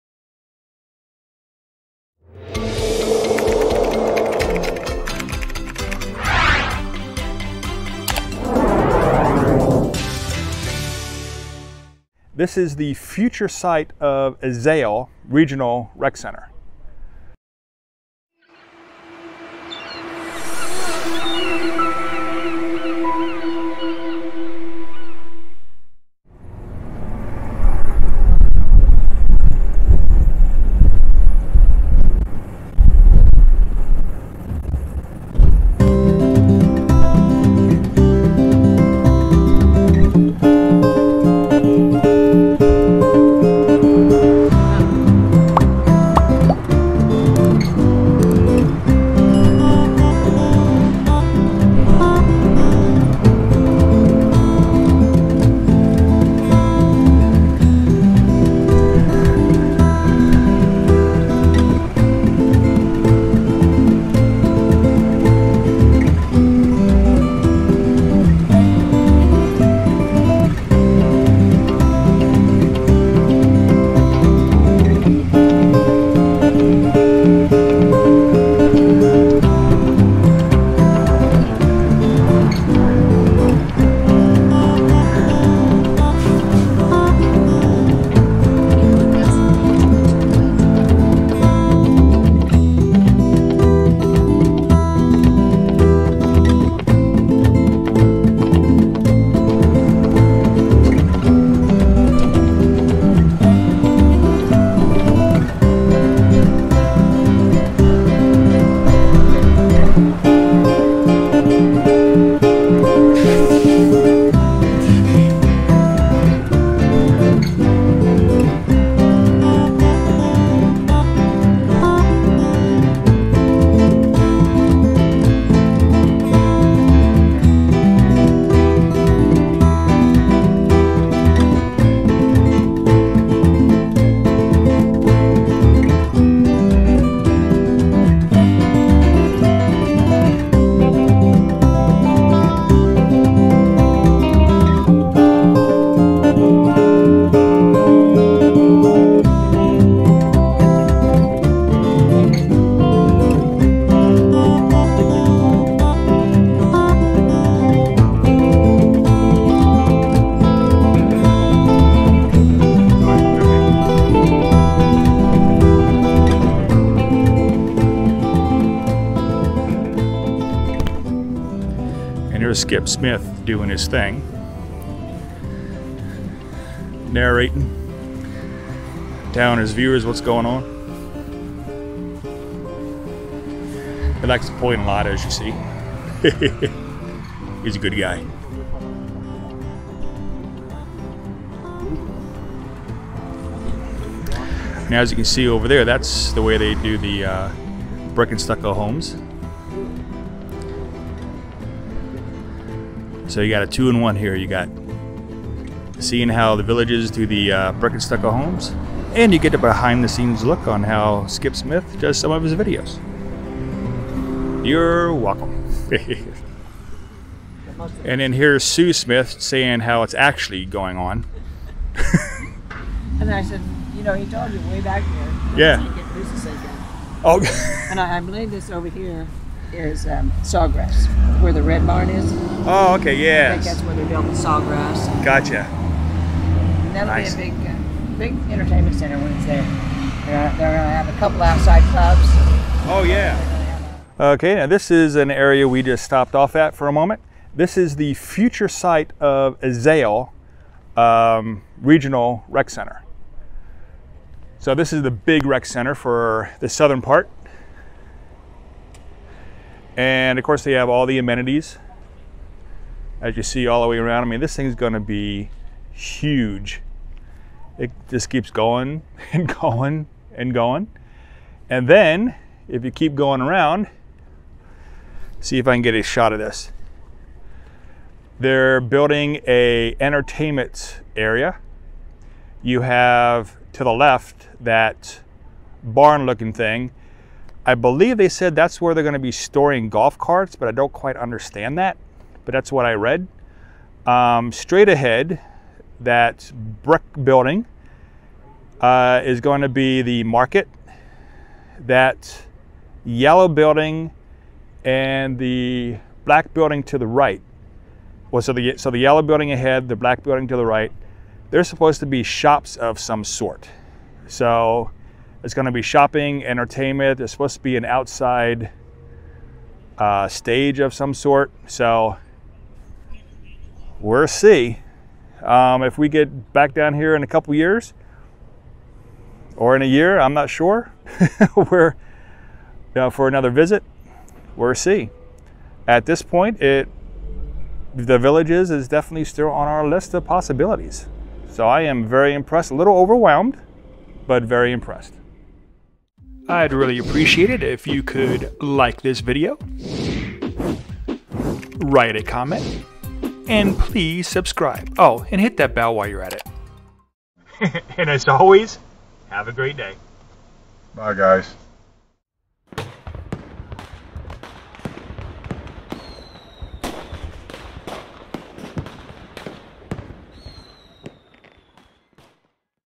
Golf Cart Ride and Ezell Regional Rec Center in THE VILLAGES Florida - Golf Car Ride
Golf Cart ride and Ezell Regional Rec Center in THE VILLAGES florida  Golf Car Ride in the villages fla

If you want to donate, to help us make more videos, you have THREE choices

YES, I am FAA Part 107 certified to fly a drone.
1) ProducerPlanet.com
2) StockNation
3) Dropmock.com